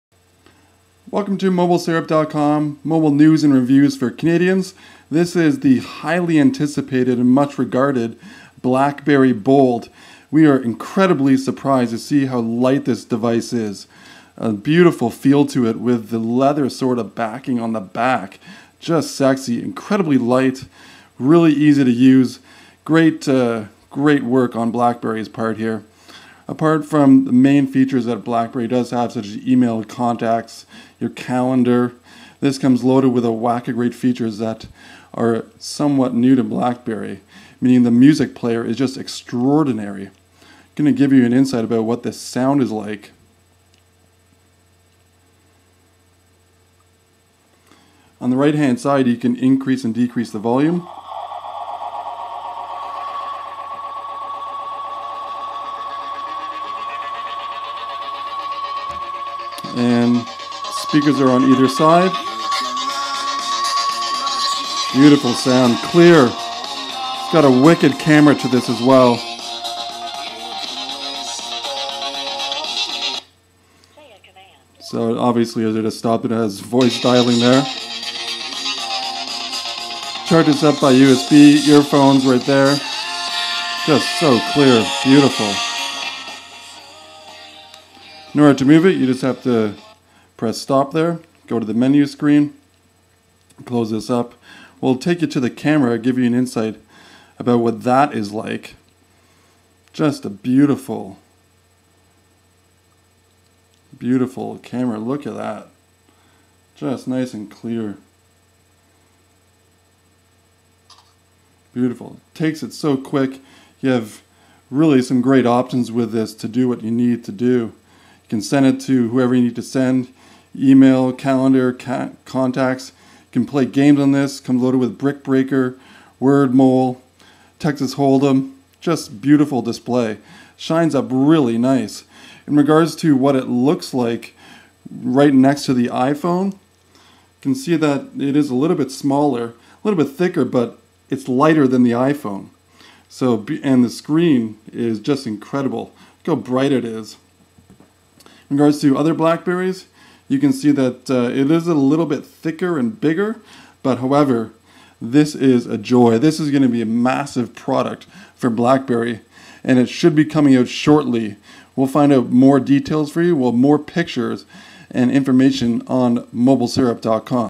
RIM BlackBerry Bold Review (Rogers Wireless)
RIM BlackBerry Bold Review (Rogers Wireless). This was our first time holding this beautiful smartphone. We will have more reviews soon